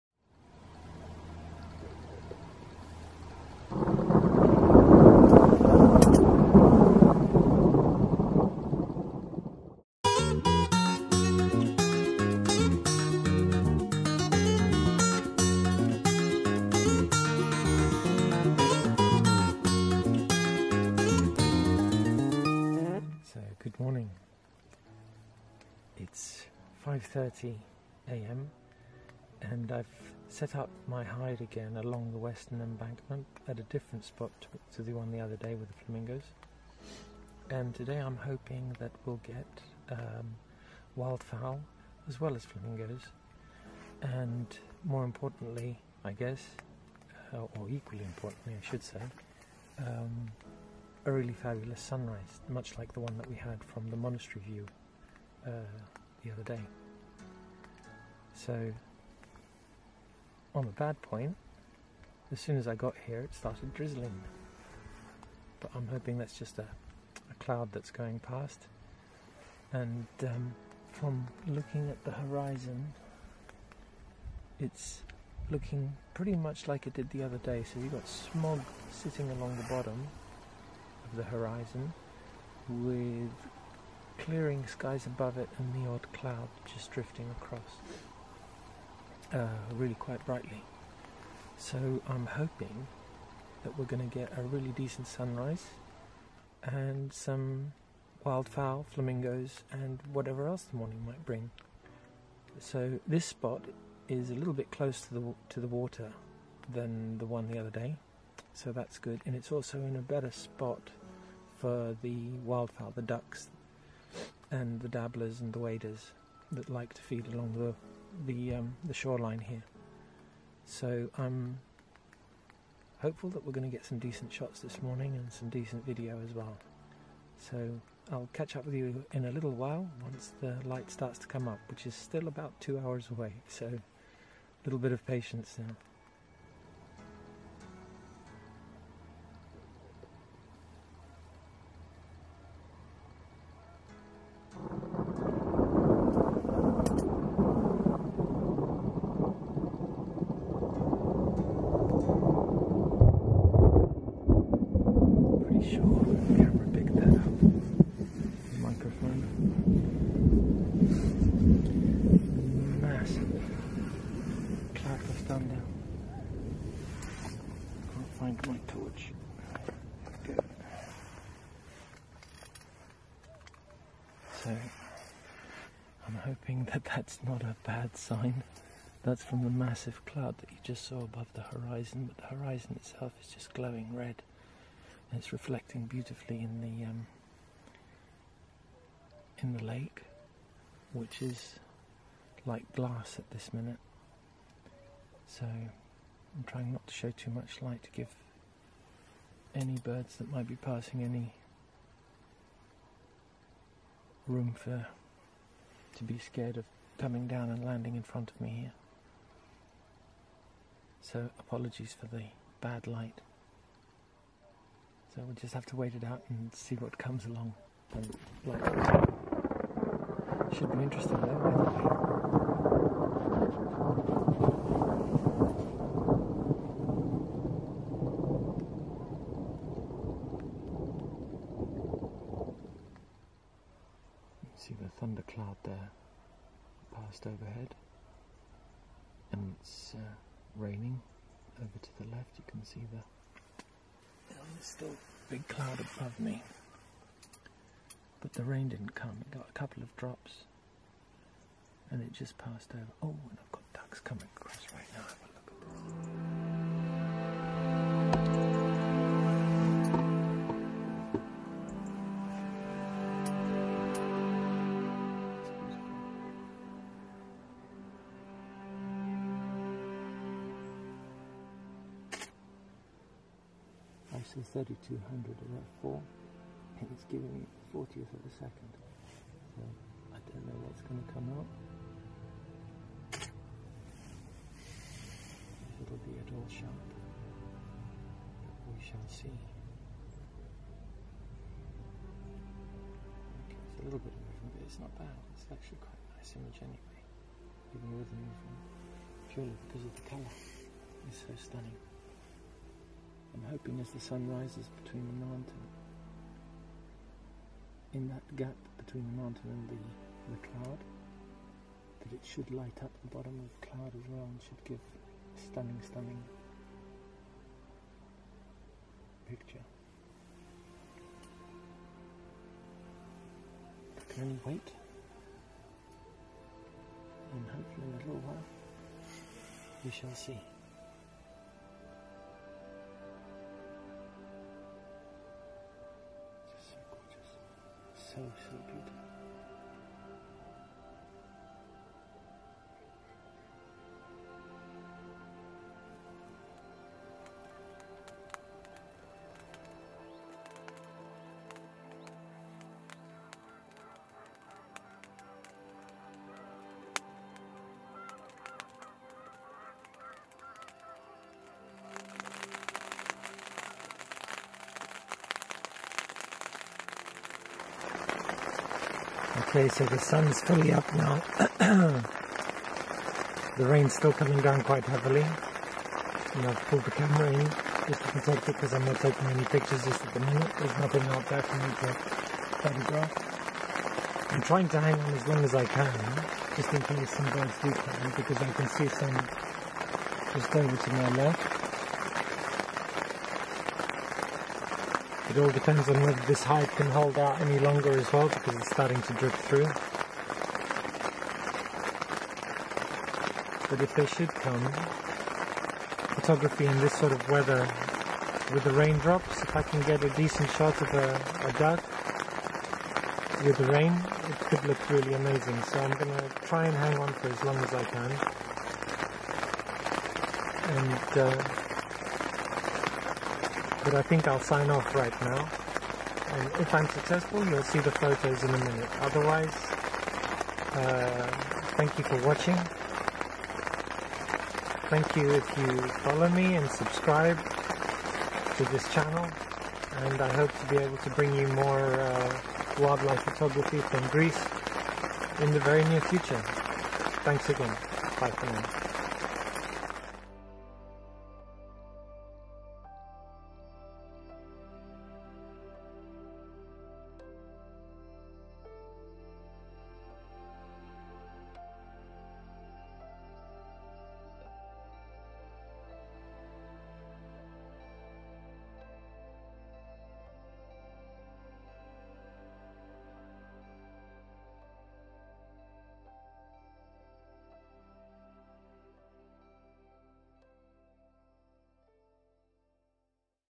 Wild Bird Photography at Lake Kerkini in Greece - Flamingos (Take 2)
Join me again in my hide on the shoreline of Lake Kerkini for some more wildlife and bird photography...

Following my "failed" attempt the other day to photograph the Flamingos at Lake Kerkini with the sunrise (due to what turned out to be a cloudy day despite the weather report predicting clear skies), this morning I decided to make a second attempt.. things started off looking good with a glowing horizon making me hopeful that a full on sunrise with flamingos and other water birds was firmly on the cards... Mother Nature though had other plans !!  A large storm cloud sat right above me thundering, lightning and raining the possibility away... I did manage to get in a few shots and just the privilege of sitting on the shoreline in my hide with the horizon glowing and the clouds thundering away above me was reward enough.. mama nature in all her glory.

best wishes
g